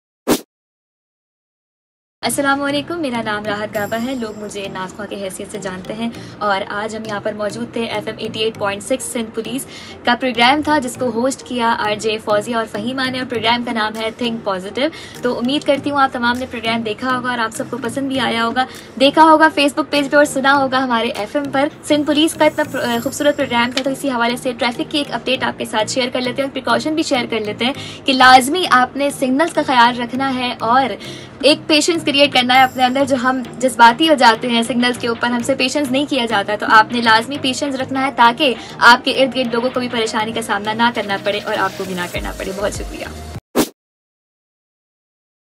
Naat khuwan "Rahat Gaba" Special video message for SPFM 88.6 listeners
Naat khuwan "Rahat Gaba"
Special video message for SPFM 88.6 listeners keep watching and like this video